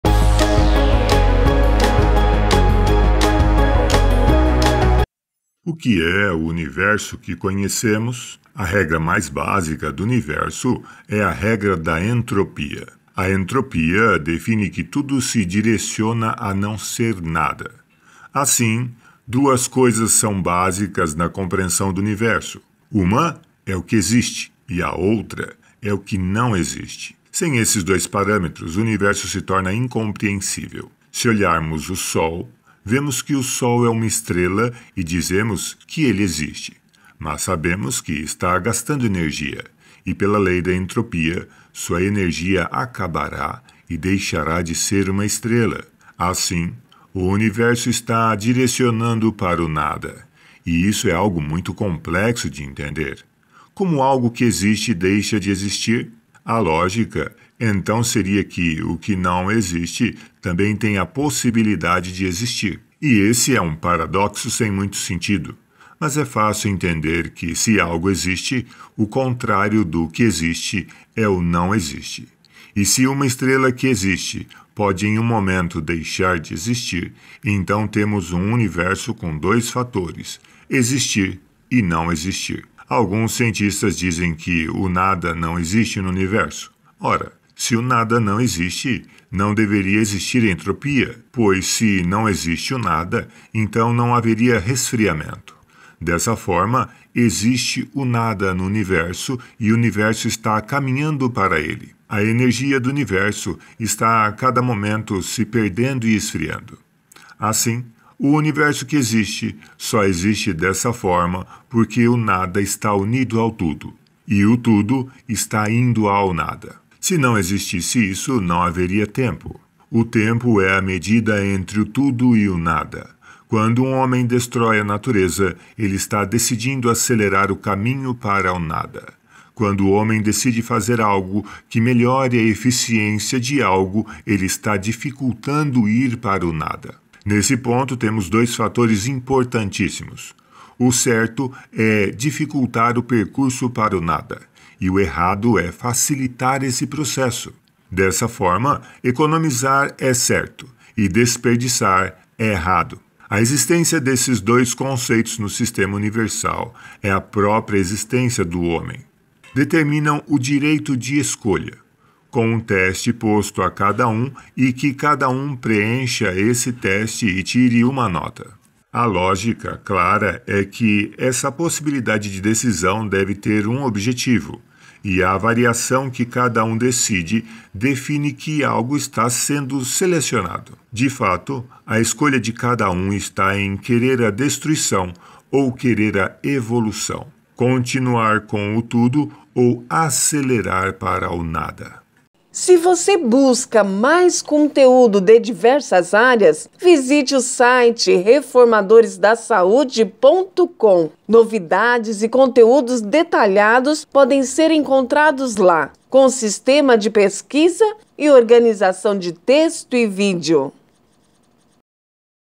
O que é o universo que conhecemos?
A regra mais básica do Universo é a regra da entropia. A entropia define que tudo se direciona a não ser nada.
Assim duas coisas são básicas na compreensão do universo. Uma é o que existe, e a outra é o que não existe. Sem estes dois parâmetros o universo se torna incompreensível.
Se olharmos o sol, vemos que o sol é uma estrela e dizemos que ele existe. Mas sabemos que está gastando energia, e pela lei da entropia sua energia acabará e deixará de ser uma estrela.
Assim, o universo está se direcionando para o nada. E isso é algo muito complexo de entender. Como algo que existe deixa de existir? A lógica, então seria que, o que não existe também tenha a possibilidade de existir.
E esse é um paradoxo sem muito sentido. Mas é fácil entender que se algo existe, o contrário do que existe, é o não existe. E se uma estrela que existe, pode em um momento deixar de existir, então temos um universo com dois fatores, existir e não existir.